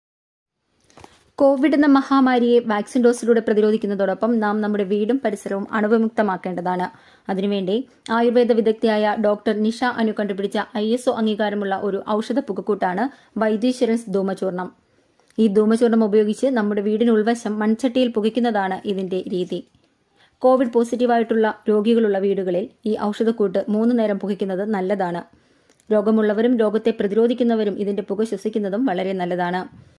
Vaidheeswaran's DHOOMA CHOORNAM
ഇരുവേലി,ദേവതരു, ജാടമഞ്ചി, കച്ചോലം, അകിൽ, തക്കോലം, തുളസി, രാമച്ചം  തുടങ്ങി 20ൽ പരം സസ്യങ്ങളുടെ ഇല, തണ്ട്, വേര്, പട്ട തുടങ്ങിയവ ഉണക്കി ചേരുംപടി ചേർത്ത് ഉണ്ടാക്കുന്ന ഈ ഔഷധ പുകക്കൂട്ട് ശരീരത്തിന് തേജസും ഓജസും നൽകുന്നതിനോടൊപ്പം പലവിധ പകർച്ചവ്യാധികളിൽ നിന്നും പ്രതിരോധിക്കുന്നു. കൂടാതെ മലിനമായ അന്തരീക്ഷം ശുദ്ധീകരിക്കുന്നതിനോടൊപ്പം ഈ ഔഷധ പുക ശ്വസിക്കുന്നത് വഴി ശരീരത്തിന് അകത്തും പുറത്തുമുള്ള  വൈറസ്, ബാക്റ്റീരിയ പോലുള്ളവയെ നശിപ്പിക്കുന്നു. ഈ അറിവ് കൂടുതൽ ആളുകളിലേക്ക് share ചെയ്യുക